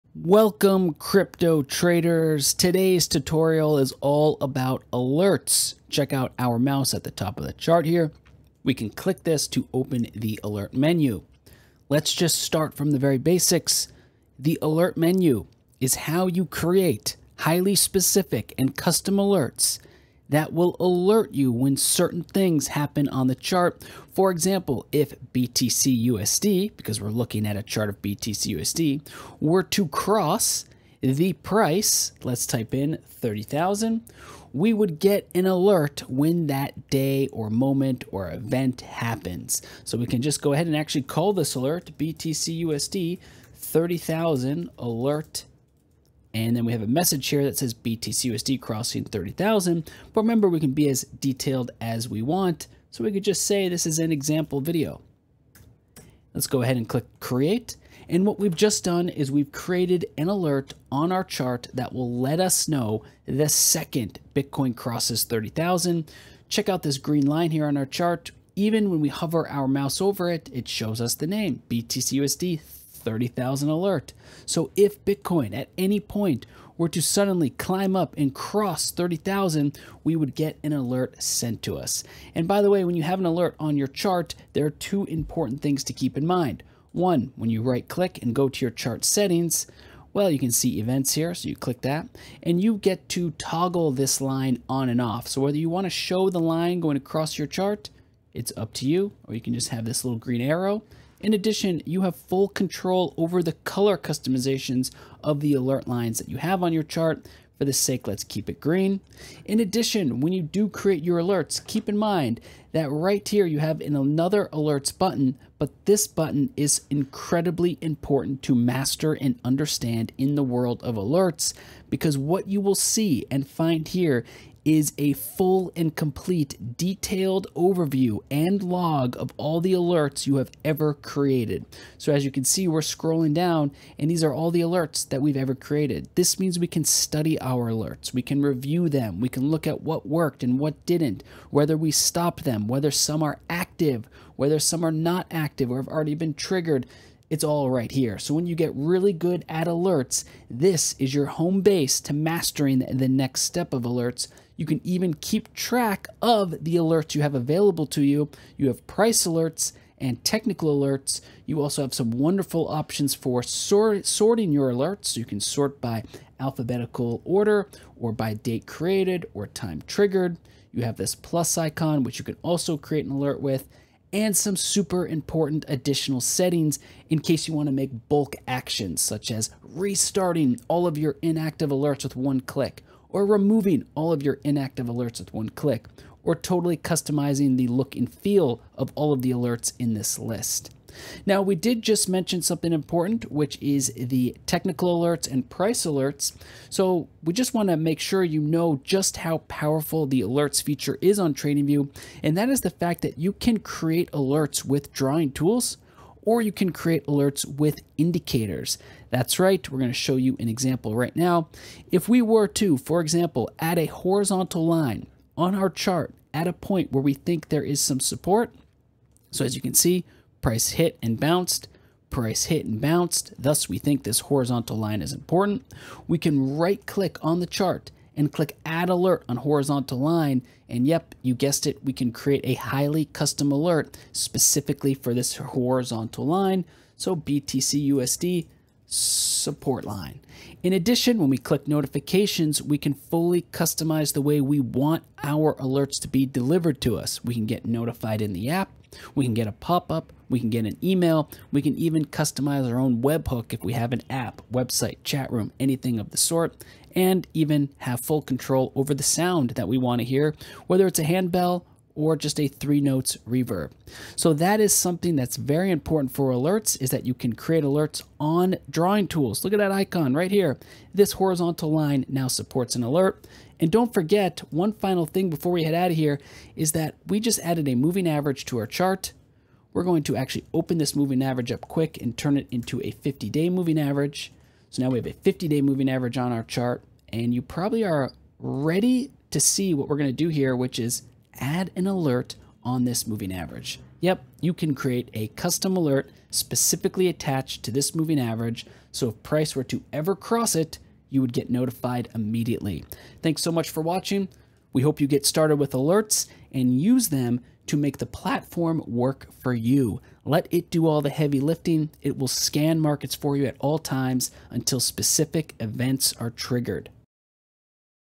How to Set Up Alerts on TradingView | Quick Guide
Learn how to set up alerts on TradingView to keep track of market movements effortlessly. This quick guide covers everything you need to know to configure price and indicator alerts, customize notifications, and manage your alerts effectively. Stay updated and try to use new knowledge on TradingView